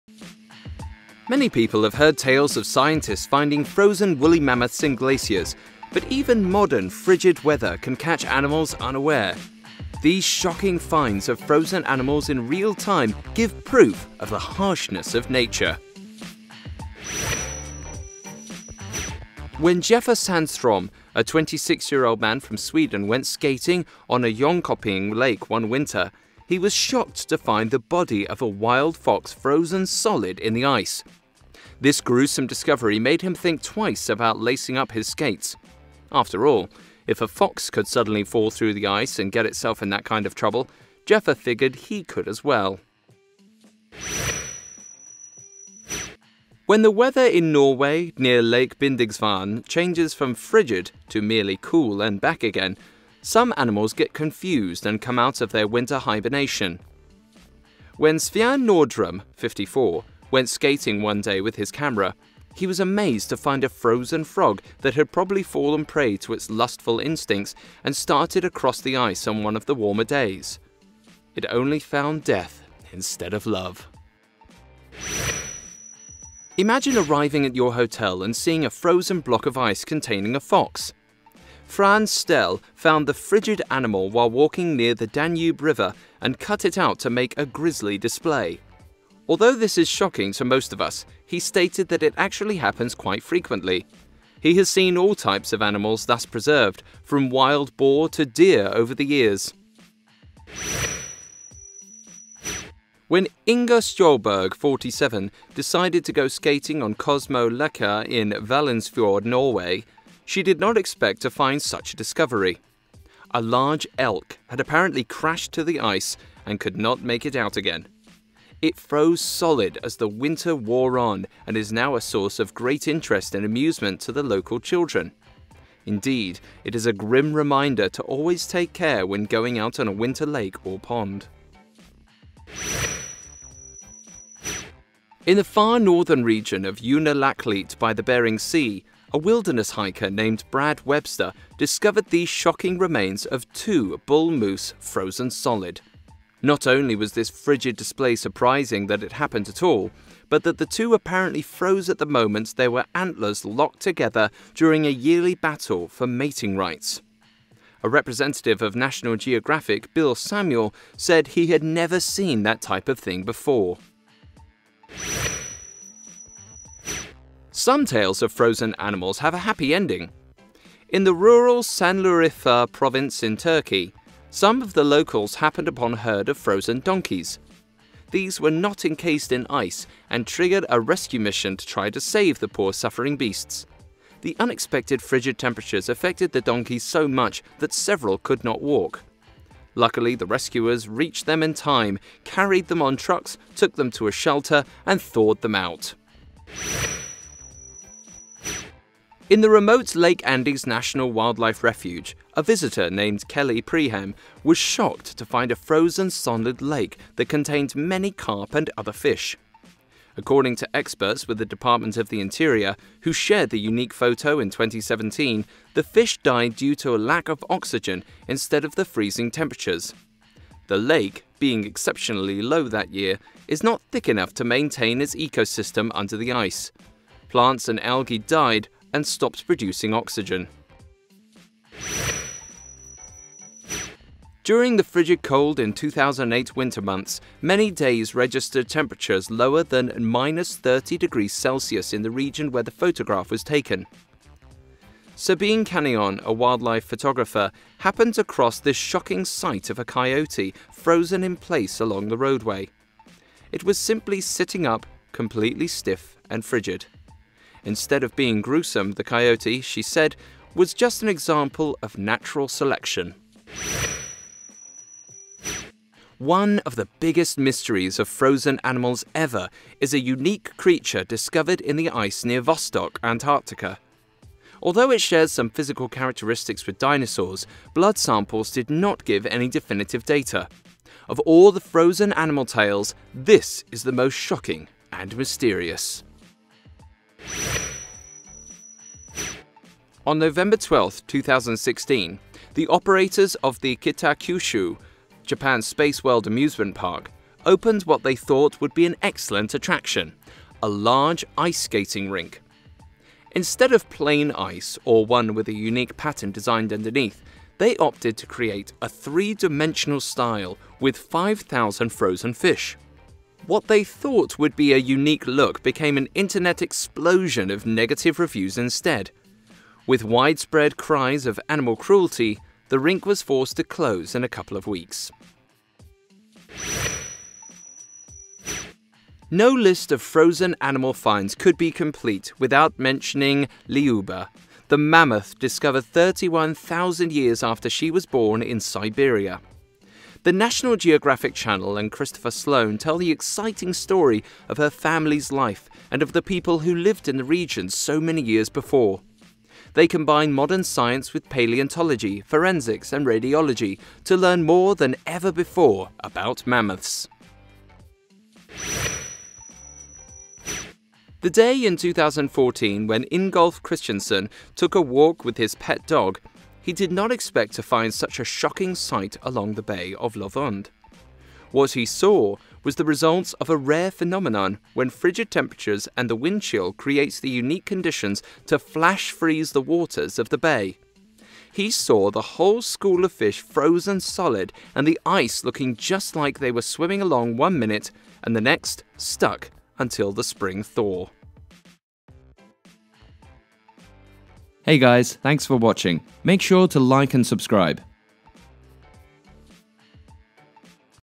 12 Animals Frozen In Real Time
Many people have heard tales of scientists finding frozen woolly mammoths in glaciers, but even modern frigid weather can catch animals unaware. These shocking fines of frozen animals in real time give proof of the harshness of nature.